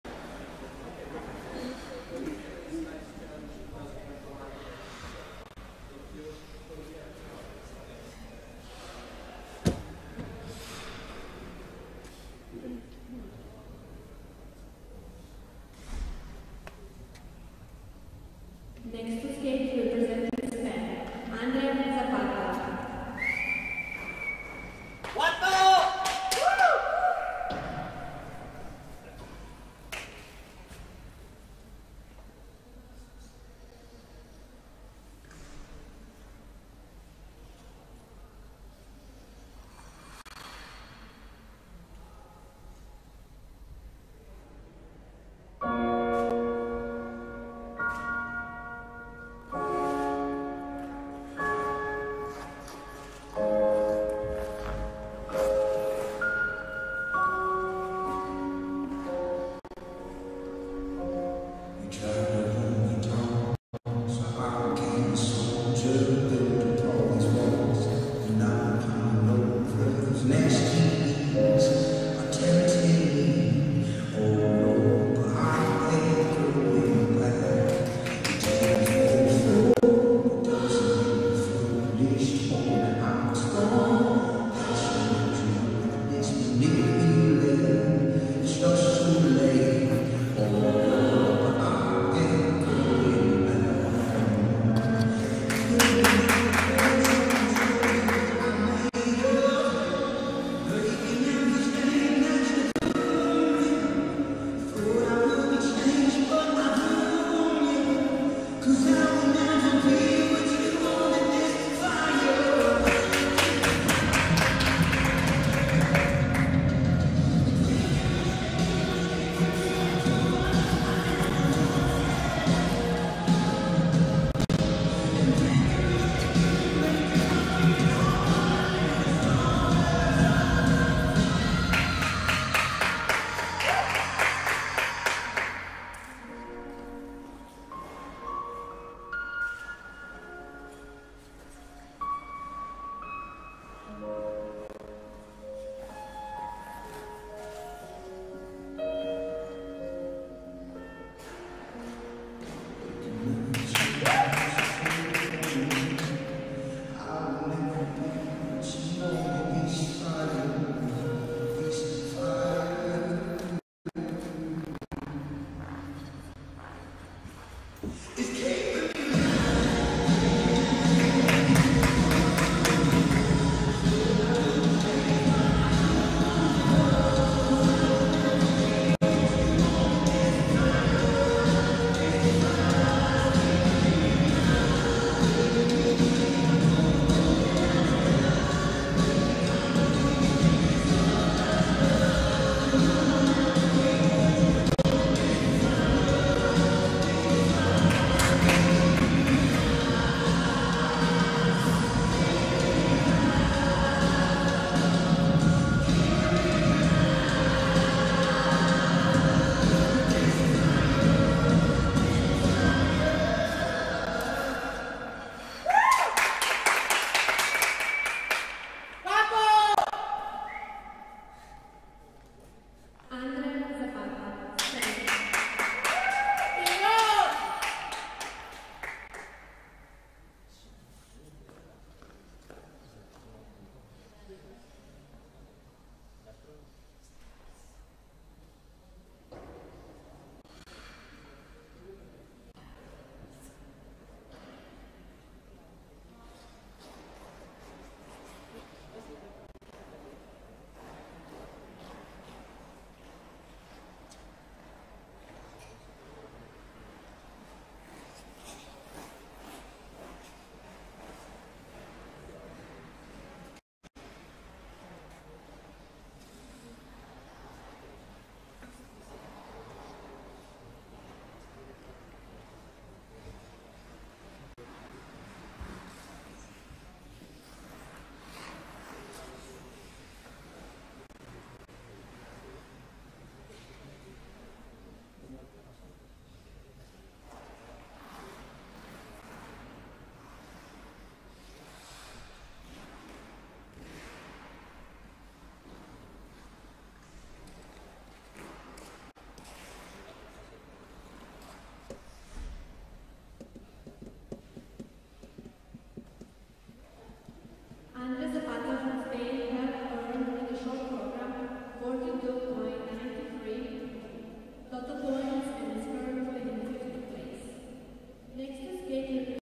Programa corto de André Zapata en el IceChallenge Next Generation 2022
Programa corto de André Zapata en el IceChallenge Next Generation 2022. Recibió una nota de 42,93 puntos finalizando 8º en la categoría júnior masculina.